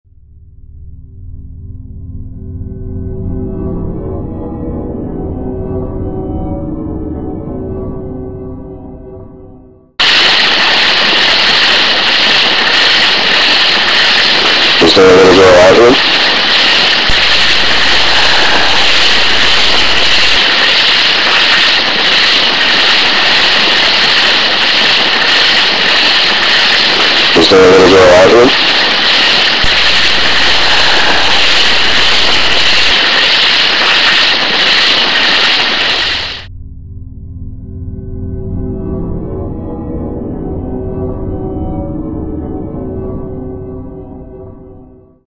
EVP at Stumpy Lake
If you listen very closeley you can hear an answer to the investigators question.